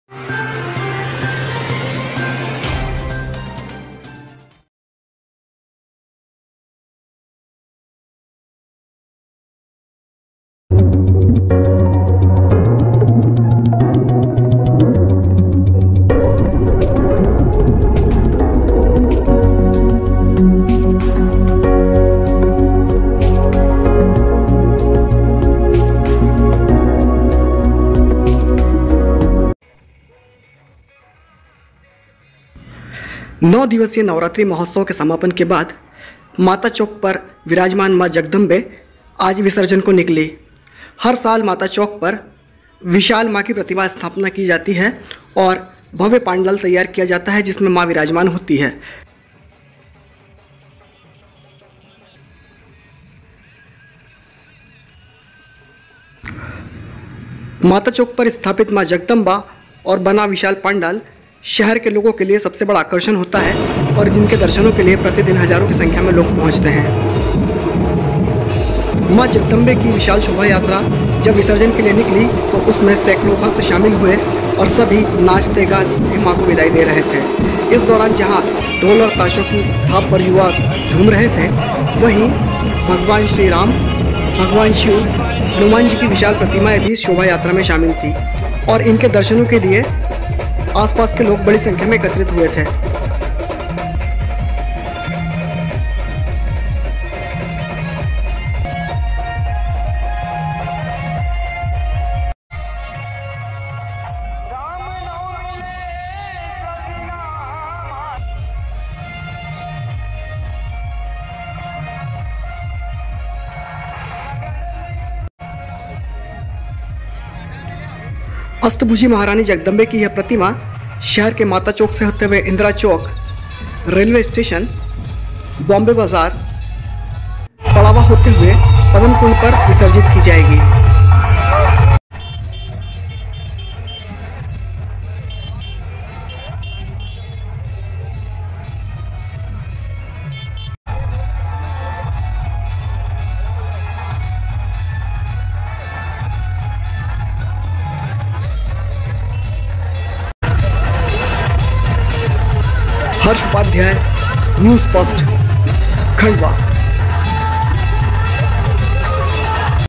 Khandwa-विसर्जन को निकली माता चौक पर विराजित जगदंबे.…..!@NewsfirstKnw
माता चौक पर विराजमान माँ जगदंबा विसर्जन को निकली  बड़ी संख्या में भक्त हुए शामिल, दर्शनों के लिए उमड़े लोग।